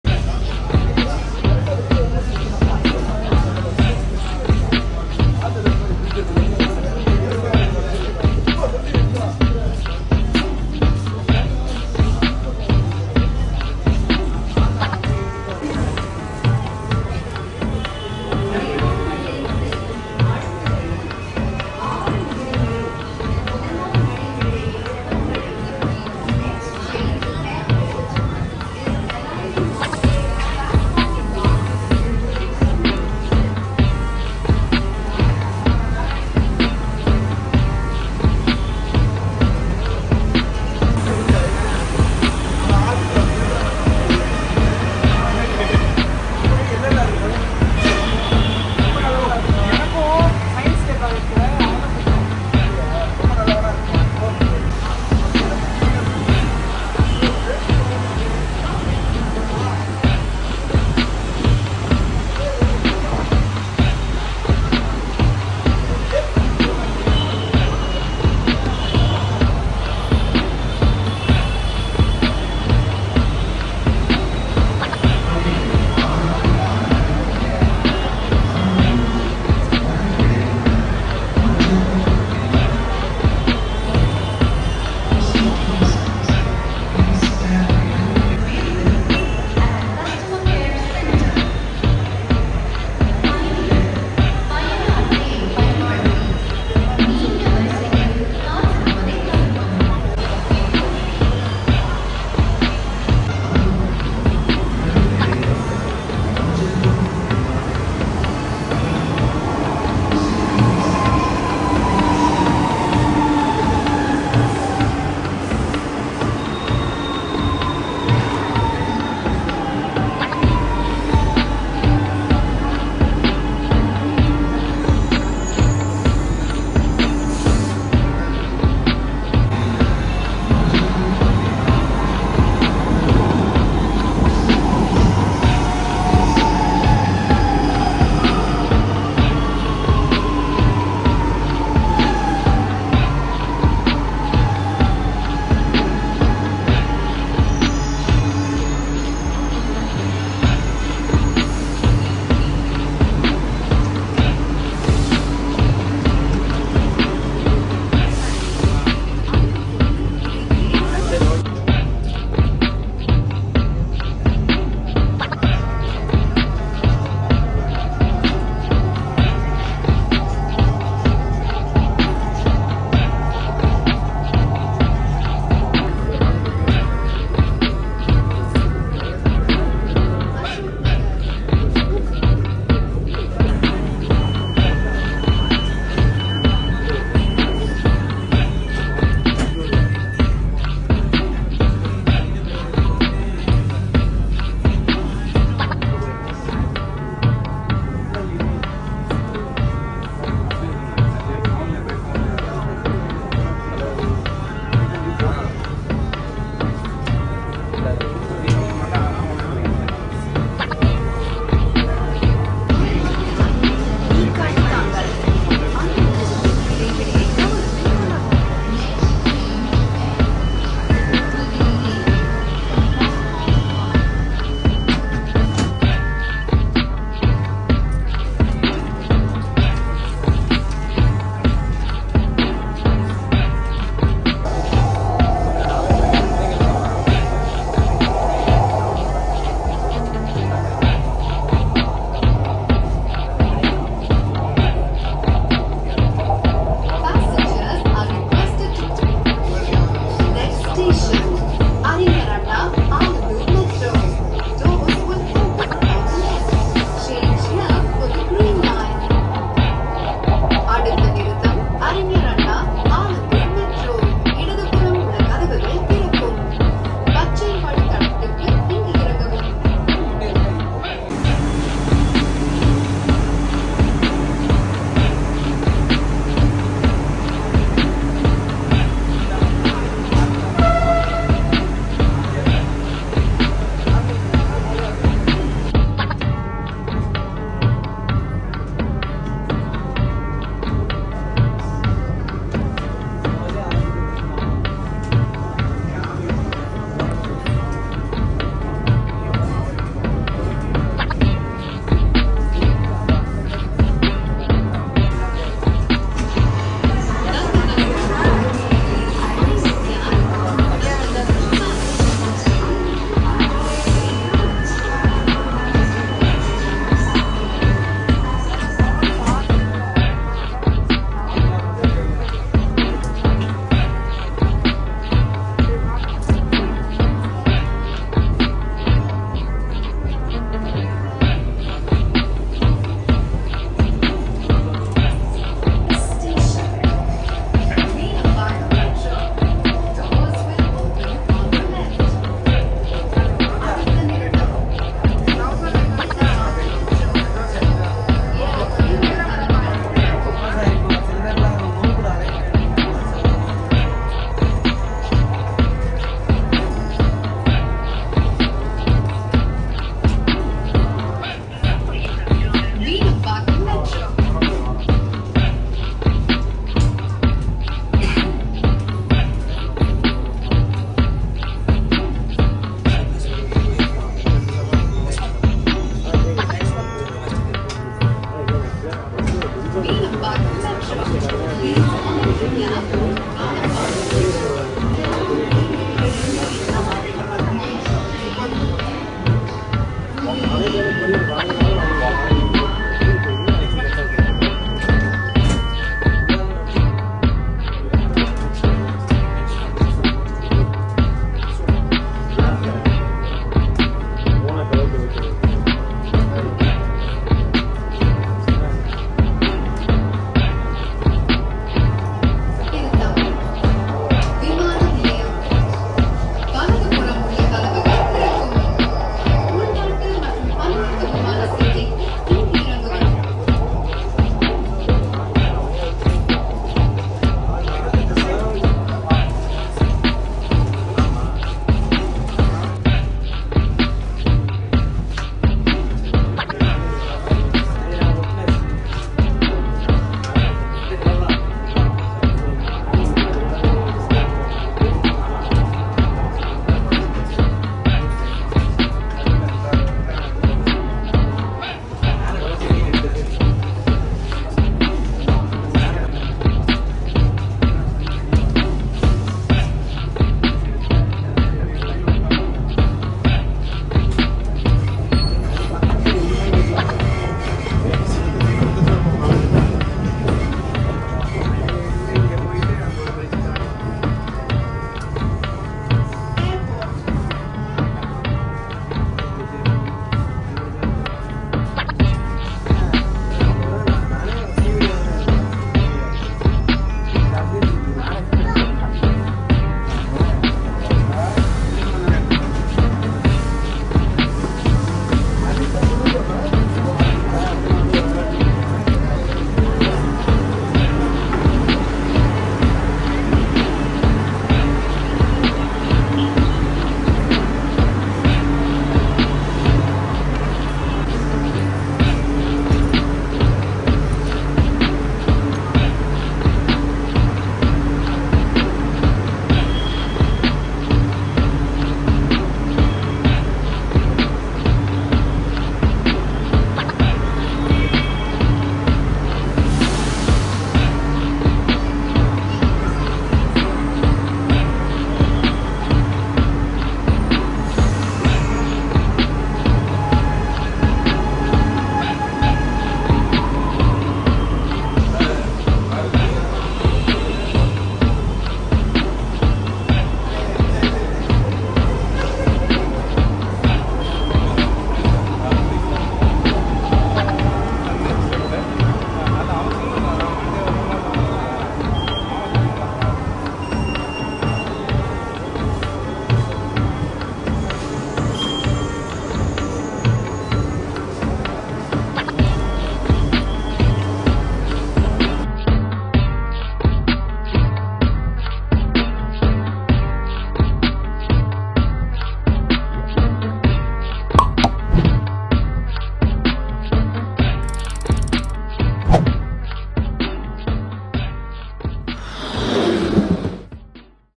Chennai Metro Rail (Vadapalani to Airport Stations), Chennai India || by: Stanlig Films
The Chennai Metro is a rapid transit system serving the city of Chennai, Tamil Nadu in India. It is said to be the 4th longest metro system in India at the moment.

You can take the MRT to and from Anna International Airport in Chennai.